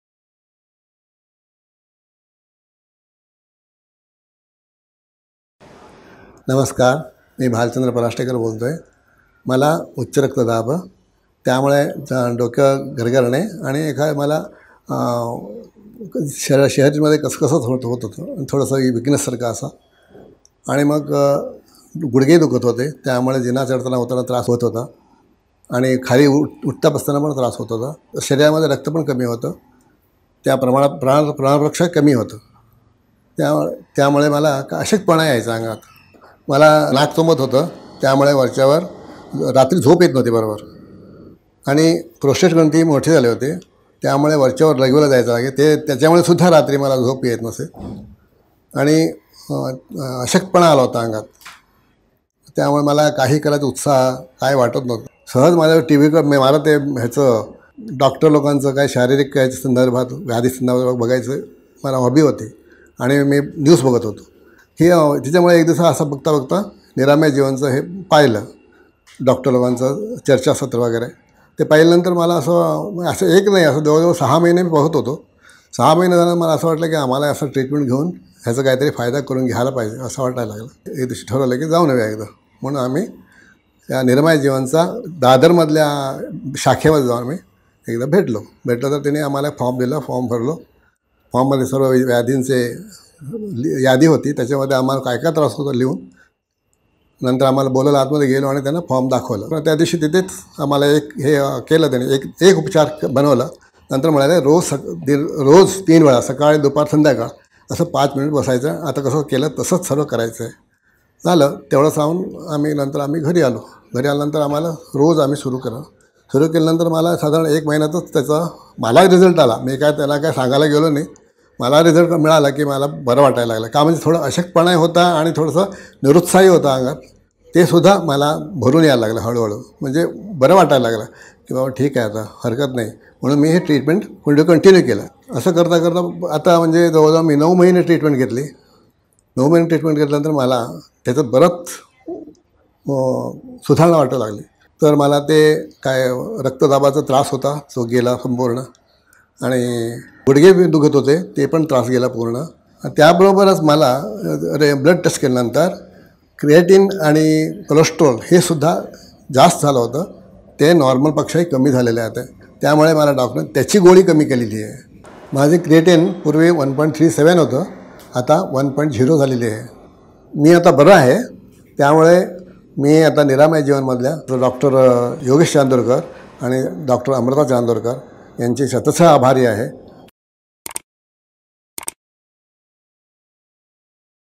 अनेक आजार एकाचवेळी दूर करणारे स्वयंपूर्ण उपचार!
भालचंद्र पराष्टेकर या ज्येष्ठ नागरिकांना अनेक त्रासांनी ग्रासले होते. उच्च रक्तदाबामुळे डोके गरगरणे, गुडघेदुखीमुळे जिने चढणे-उतरण्यास त्रास, अंगात रक्त कमी असल्यामुळे अशक्तपणा, नाक चोंदल्यामुळे रात्री झोप न येणे, लघवीला वारंवार जावे लागणे या त्रासांमुळे ते त्रस्त व निरूत्साही झाले होते. साम टिव्हीवरचा निरामयचा कार्यक्रम पाहून ते दादरच्या निरामय क्लिनिकमध्ये आले. त्यानंतर त्यांनी ट्रीटमेंट घ्यायला सुरूवात केली. एका महिन्यातच त्यांना फरक जाणवला. त्यामुळे विश्‍वासानं त्यांनी ट्रीटमेंट पुढे सुरू ठेवली. 9 महिन्यातच त्यांच्यामध्ये खूप सुधारणा दिसू लागली. उच्च रक्तदाब कमी झाला, गुडघेदुखी कमी झाली. ब्लड रिपोर्टमध्ये क्रिएटीनची लेव्हल नॉर्मल आली. त्यामुळे ते आता आनंदाने जीवन जगत आहेत.

विविध आजारांवर यशस्वीपणे मात करणारा हा अनुभव ह्या व्हिडीओमध्ये पाहा आणि इतरांनाही शेअर करा.